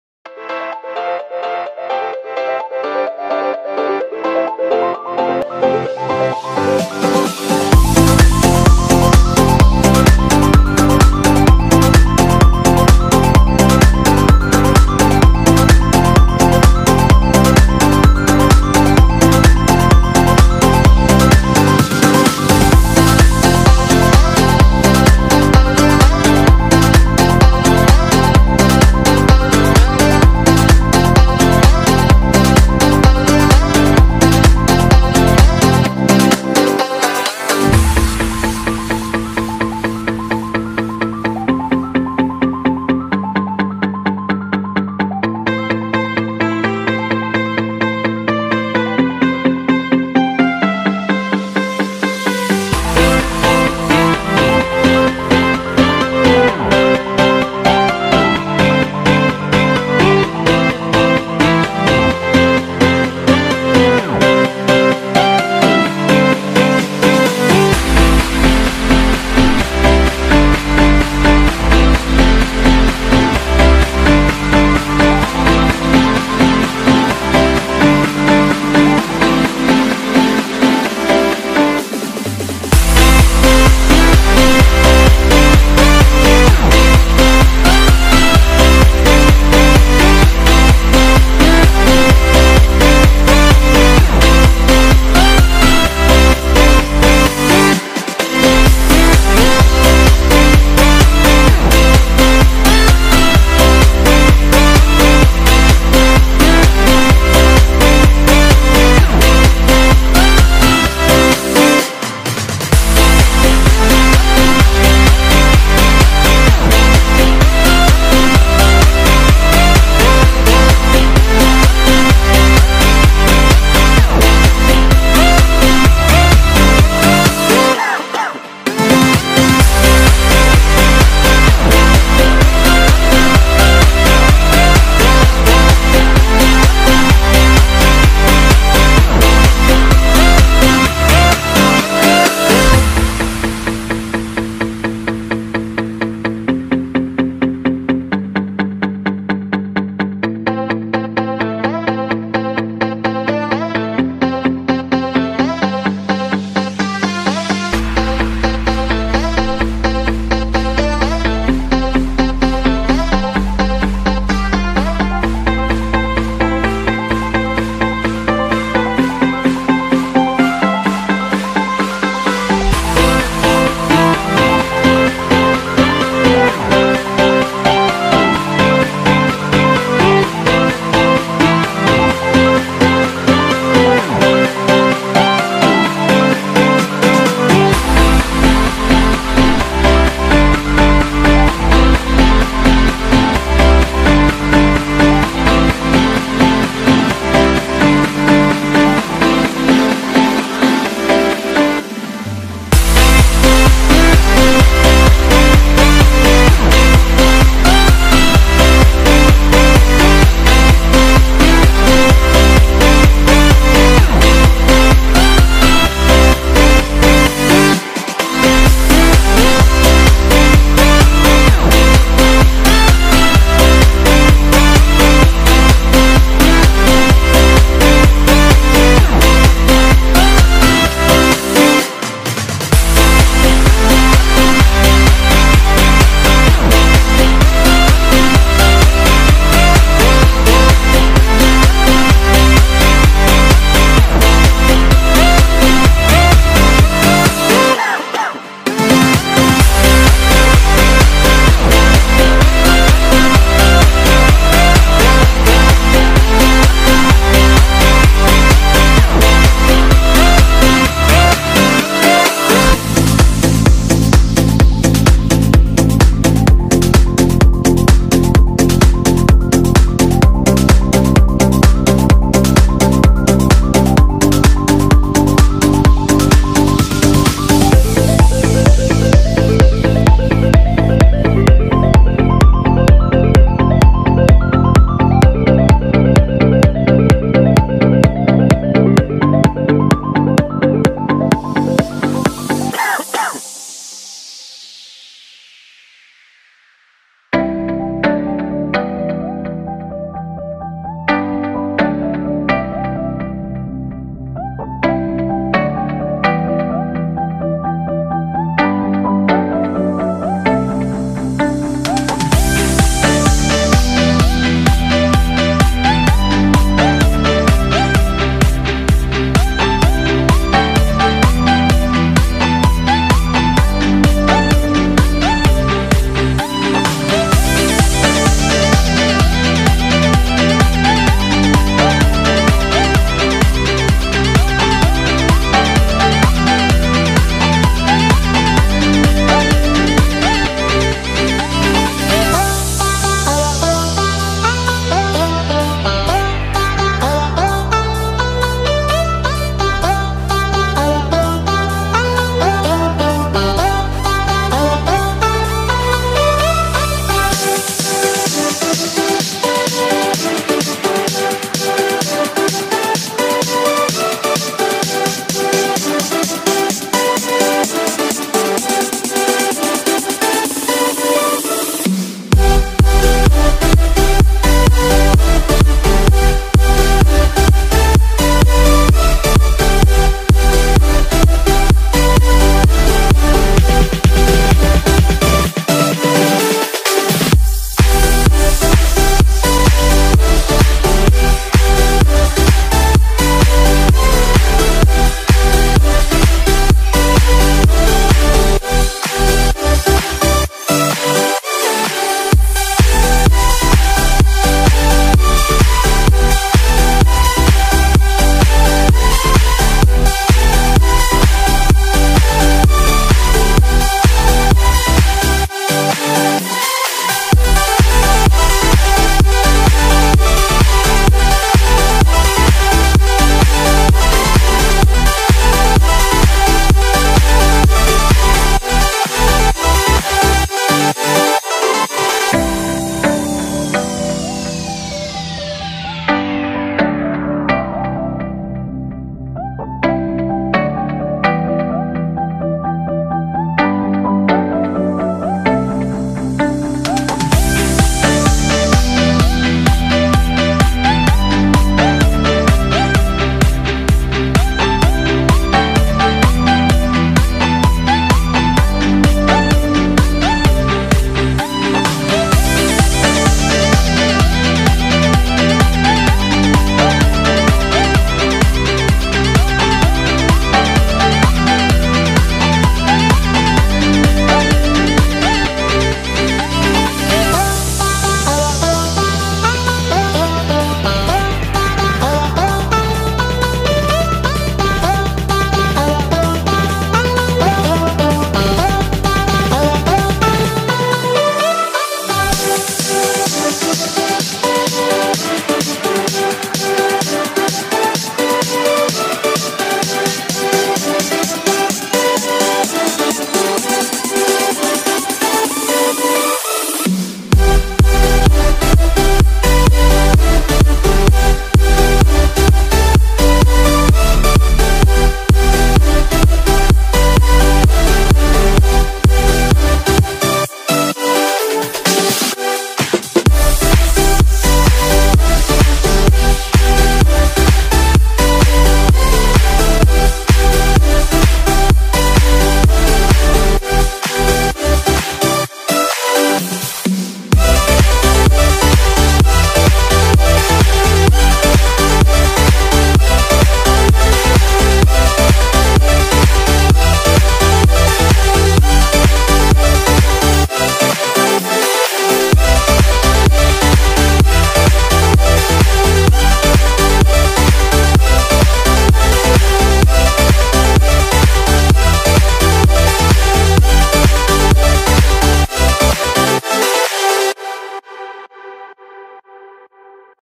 [SpeedPaint] cute furry on android - autodesk sketchbook
SpeedPaint on android phone - finger drawing(digital draw).
digital art speed painting on smartphone.
SpeedPaint furry on smartphone.
Speedpaint furry
Speedpaint cute furry
Speedpaint furry kawaii